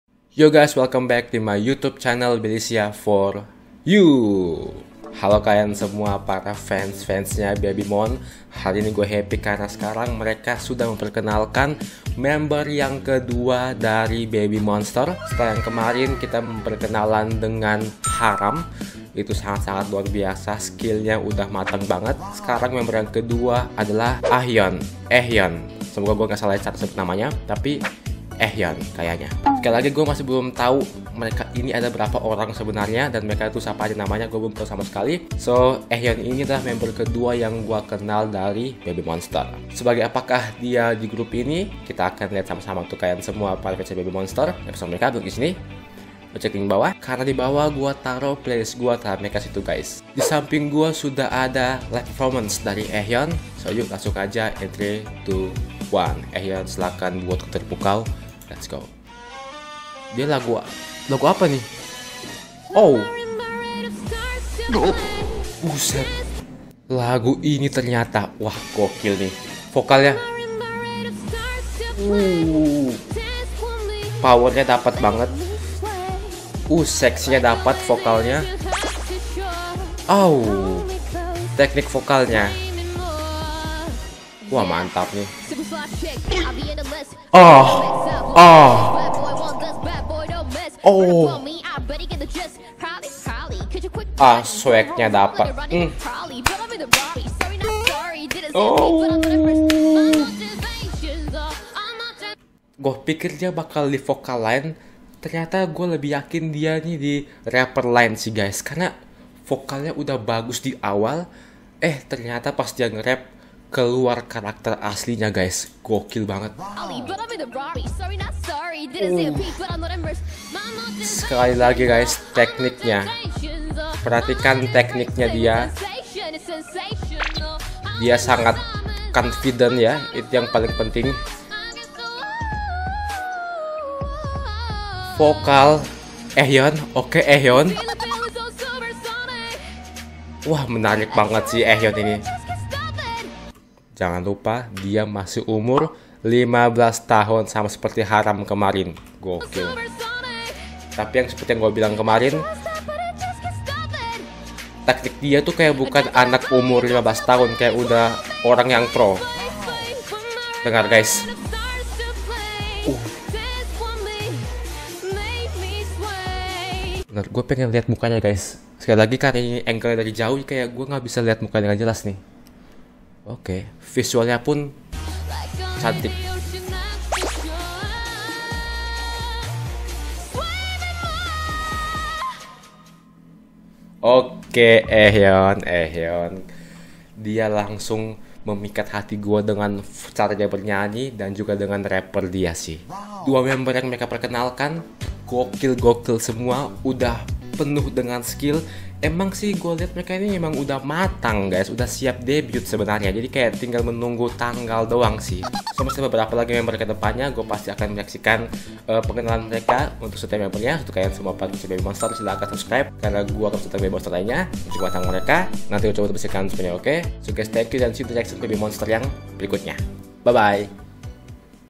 SKILL-NYA UDAH MATANG BANGET🔥|| BABYMONSTER (2) - AHYEON (Live Performance) || REACTION (Indonesia)
~Gue jadi semakin penasaran sama member-member grup ini~